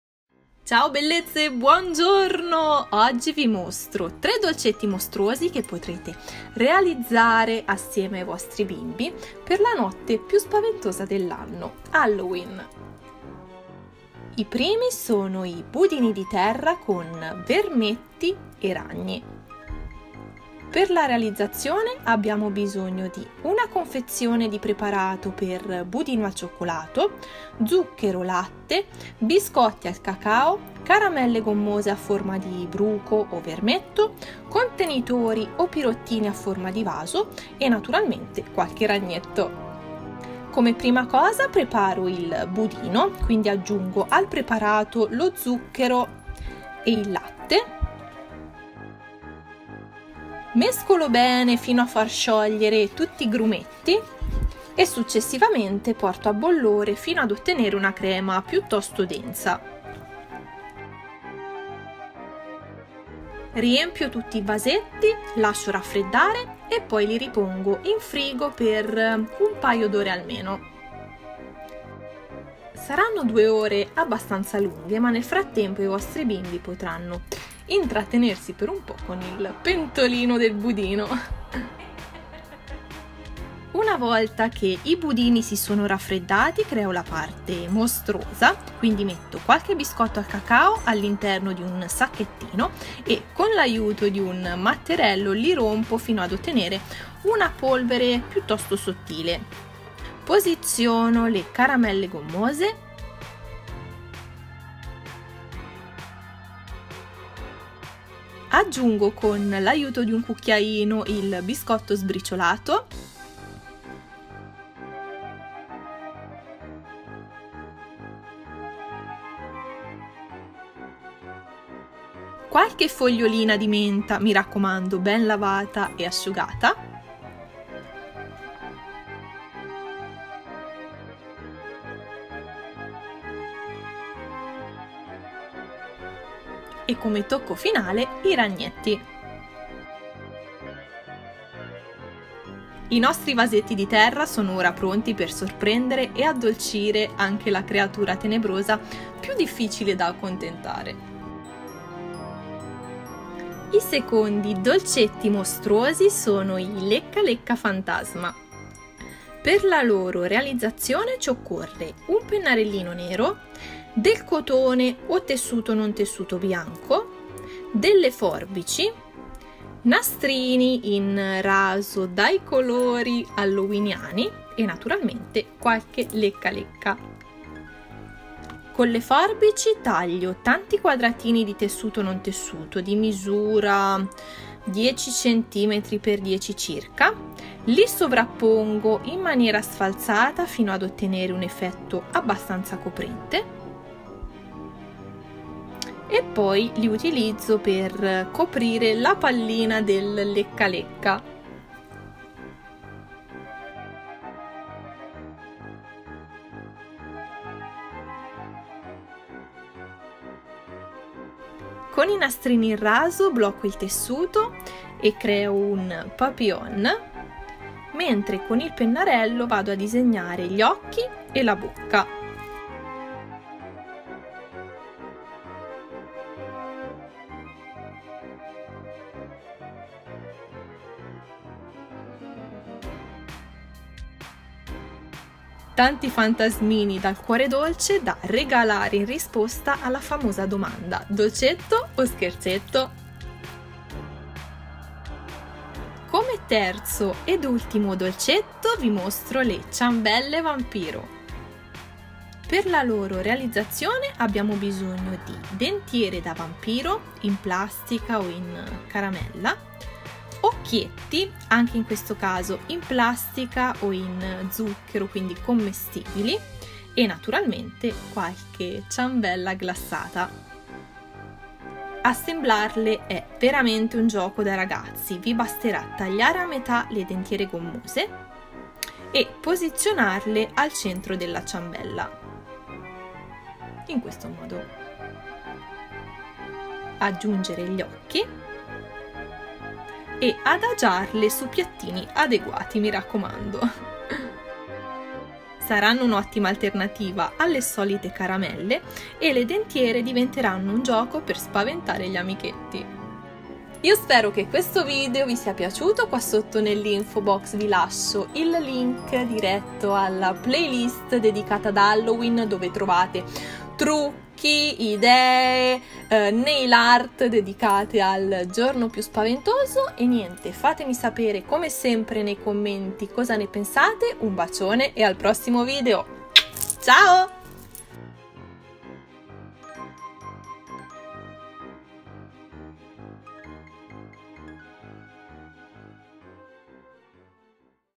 3 DOLCETTI mostruosi per HALLOWEEN - DIY facili per BAMBINI
Ciao bellezze! Buongiorno!
Halloween si avvicina e noi non ci facciamo sorprendere impreparate: Oggi prepariamo insieme dei dolcetti mostruosi!
3 idee facili e veloci da riprodurre, anche in compagnia dei nostri bimbi!

♥ SNAPCHAT (come Ilaria Costa o YlyStyle)